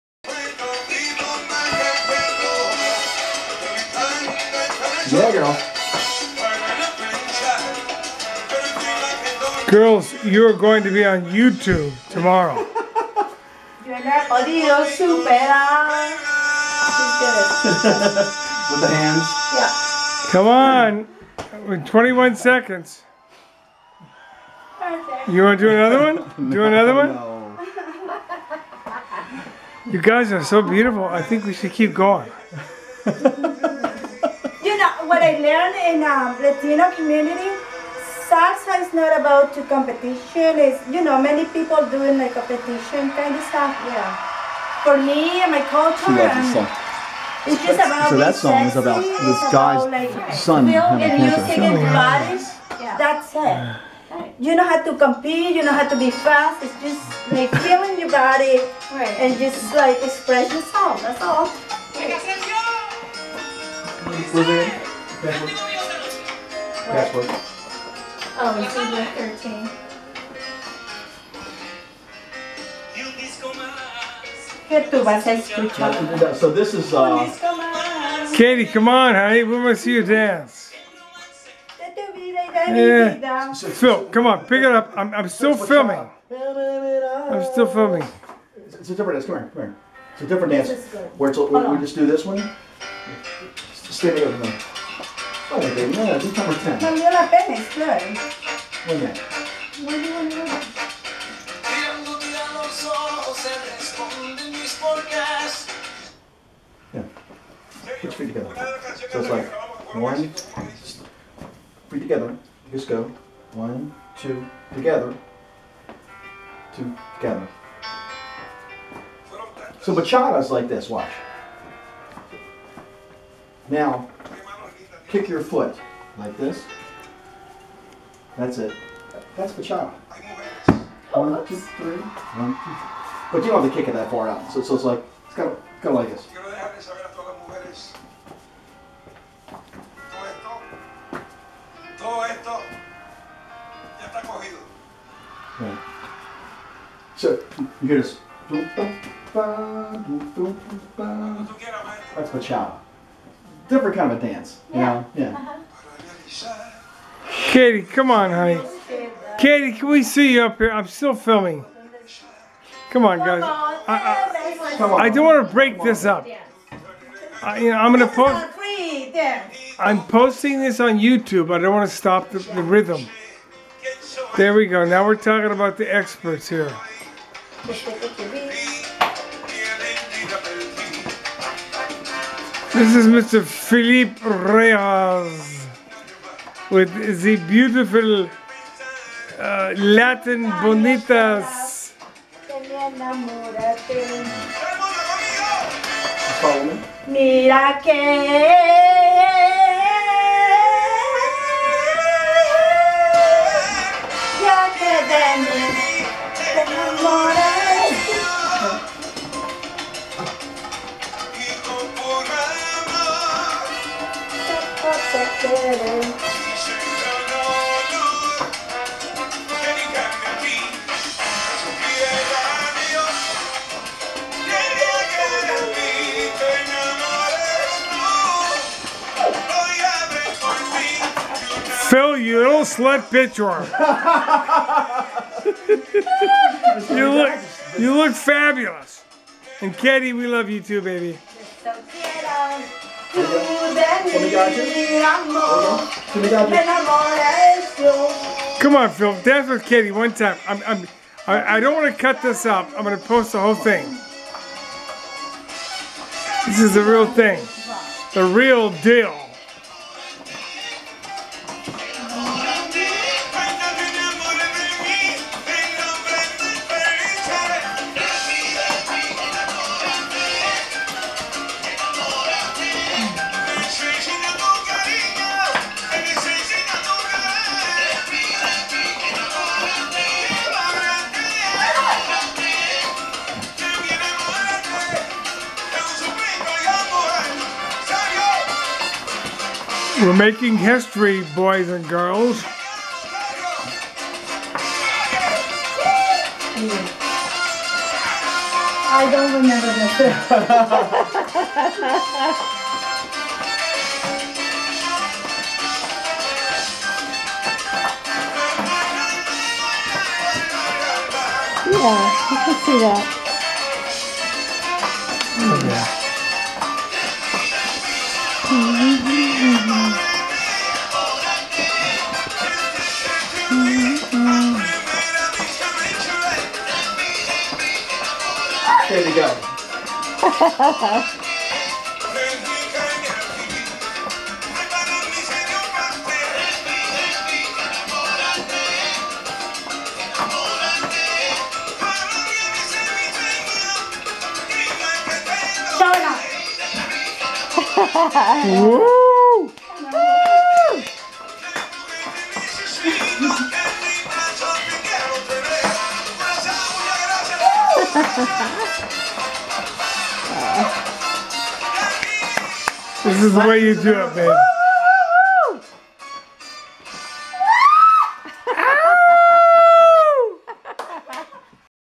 Salsa Dance Instructor, Sir Phil, and The Oh La Latina Girls
Salsa Instructor, AKA Mambo King Phil, Shows the Lady's, Oh La Lonnee and Queen Katie, how they cut it up South of the Border.